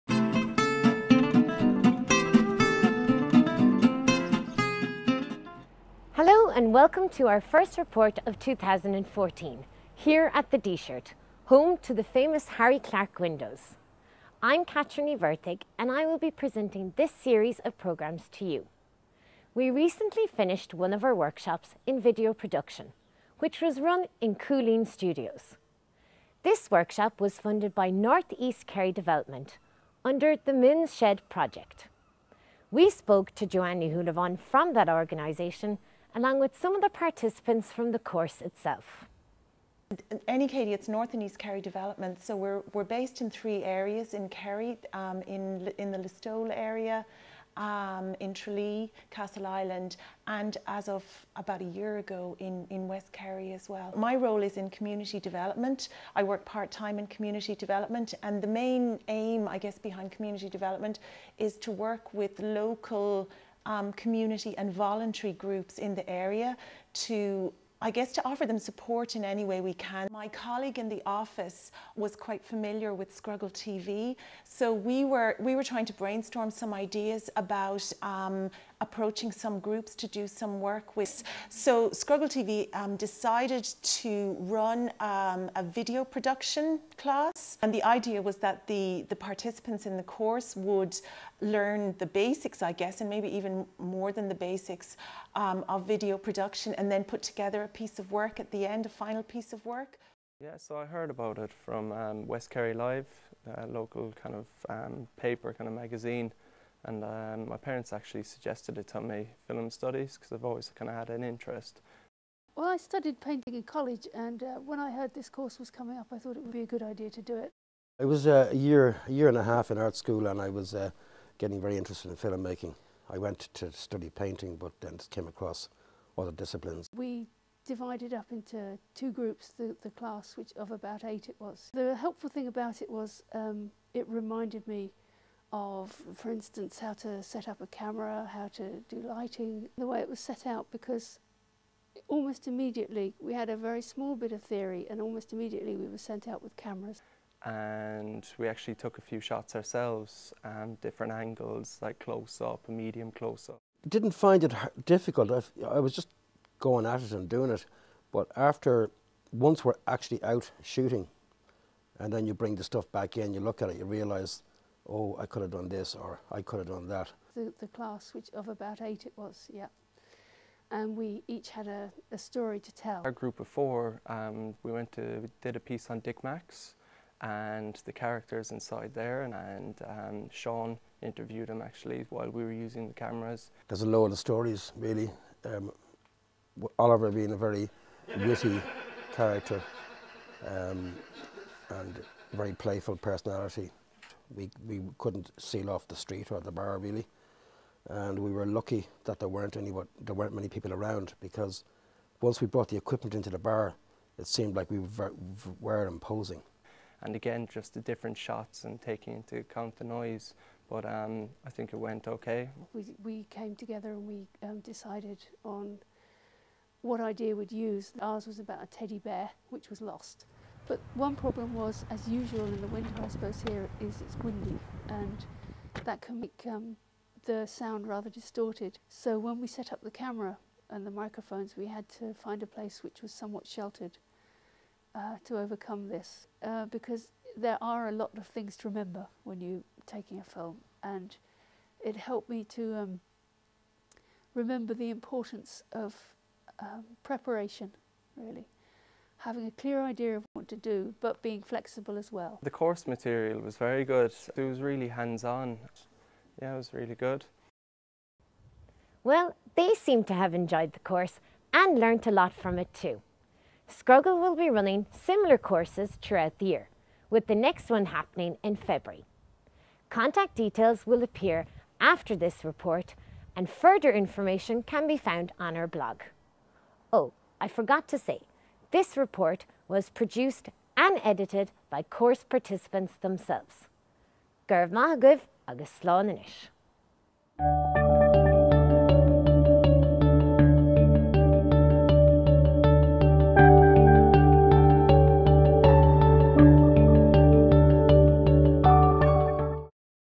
Cúrsa Treanála - Training course
Tugann rannpháirtaithe de cheardlann i Léiriúcháin Físe, a bhí á rith ag Scrogall TV,cur síos ar a dtaithí féin
Students of a Video Production workshop run by Scrogall TV give an account of their experiences during the course.